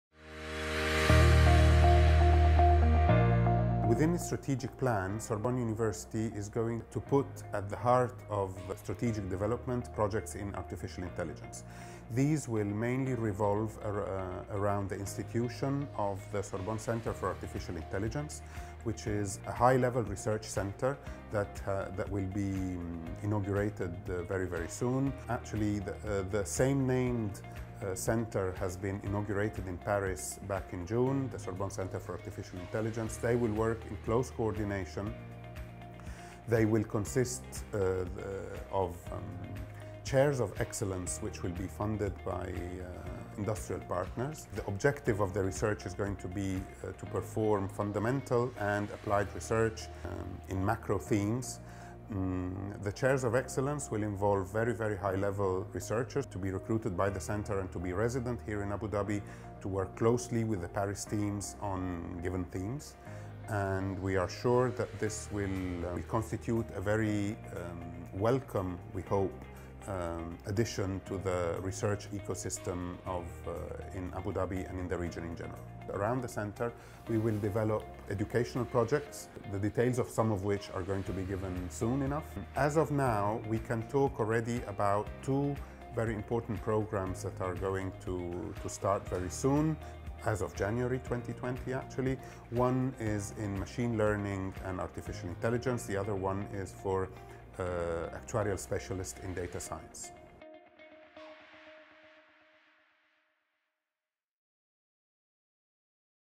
Sorbonne University Abu Dhabi to offer AI courses
Sorbonne University Abu Dhabi is setting up an AI hub as part of its strategic plans